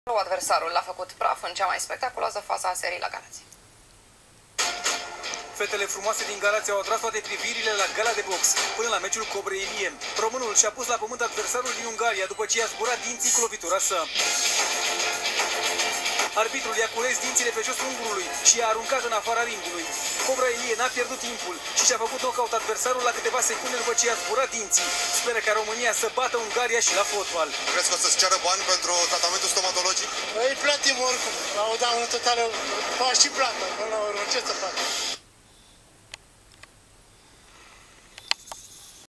Giulian Cobra Ilie ugly KO !!! WOW!
Boxing...You have to see it to believe it! teeth flying away ... referee picks them up and throw away!! KO round 1